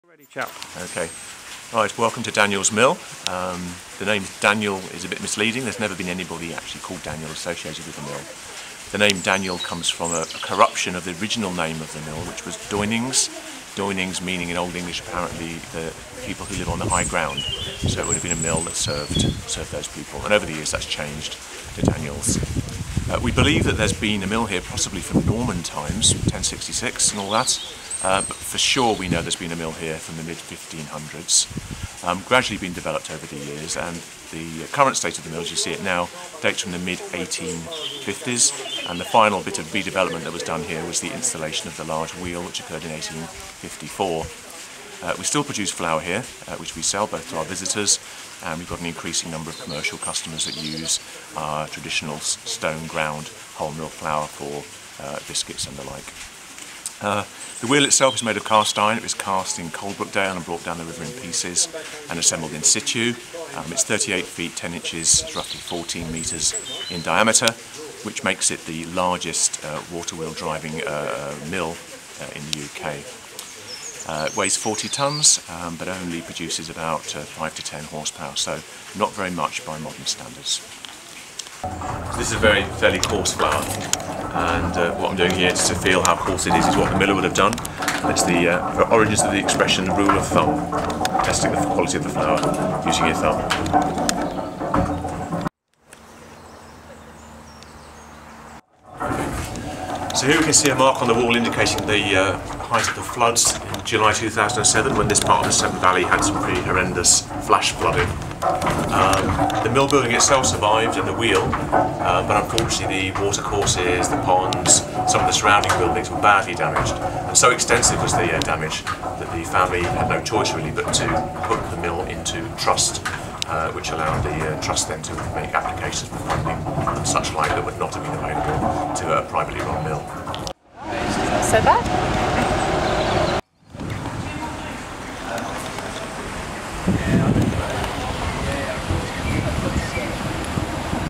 Major restoration planned at Daniels Mill, Bridgnorth
Daniels Mill in Bridgnorth, the largest working mill waterwheel in the UK, is set for a major restoration in a project expected to cost more than £1.5 million.